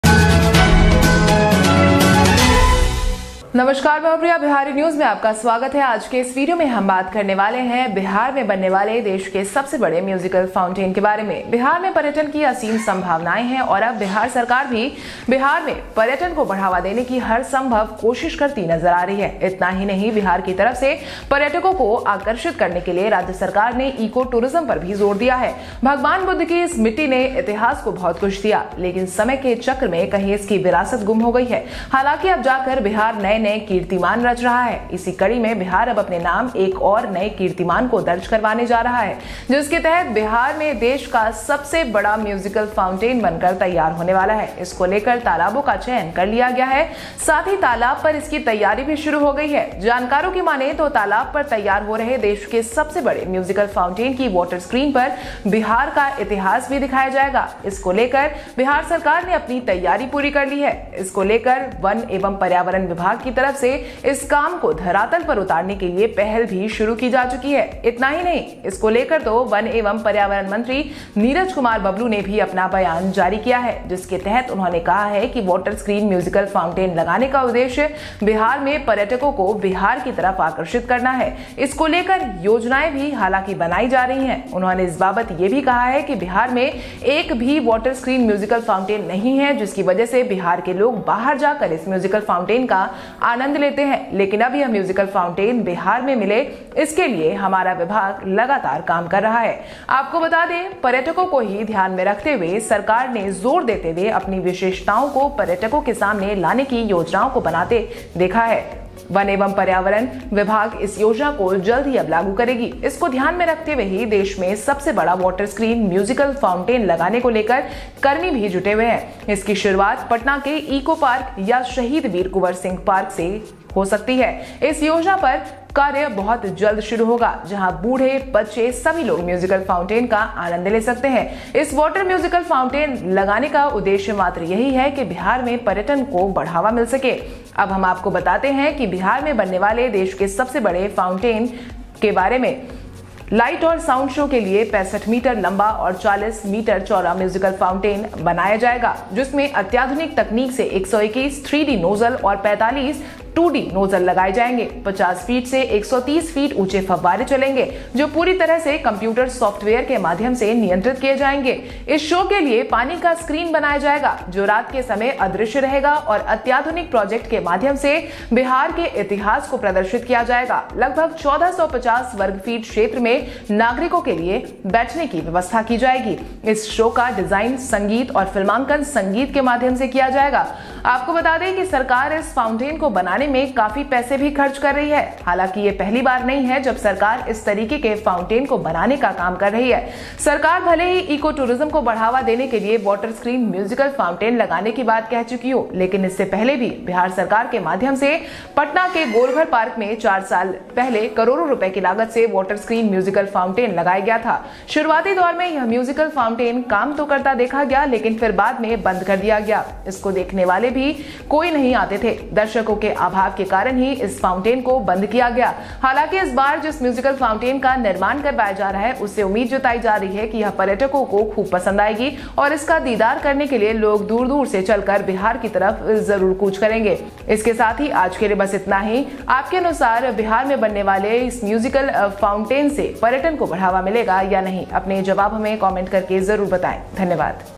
बिहार में देश के सबसे बड़े म्यूजिकल फाउंटेन बनाने की शुरू हुई तैयारी.Bihar Largest Musical Fountain.
In Bihar, the country's largest musical fountain is going to be ready, Talab has been selected for this, as well as preparations have started on the pond.
The history of Bihar will also be shown on the screen, the Bihar government has completed its preparations, for this, the initiative has been started by the Forest and Environment Department to take this work to the surface.